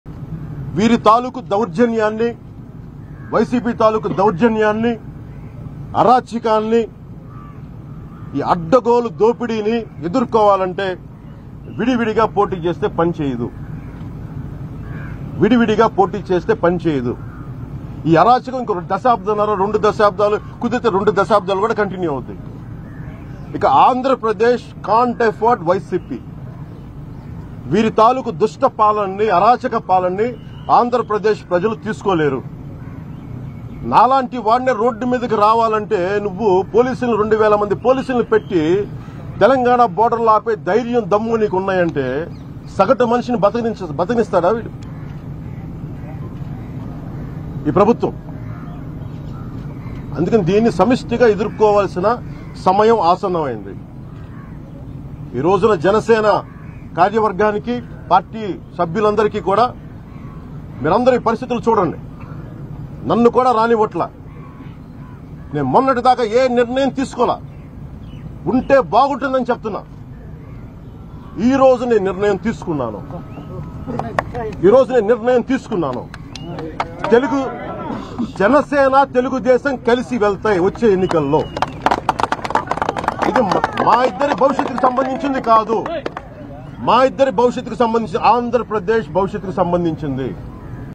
Pawan Kalyan About JanaSena TDP Alliance in 2024 Elections in Rajahmundry | AP Politics
2024లో జనసేన -టీడీపీ పొత్తు ఉంటుంది. JanaSena party president Pawan Kalyan confirmed alliance with TDP in 2024 elections. Pawan Kalyan met Chandrababu Naidu in Rajahmundry central jail.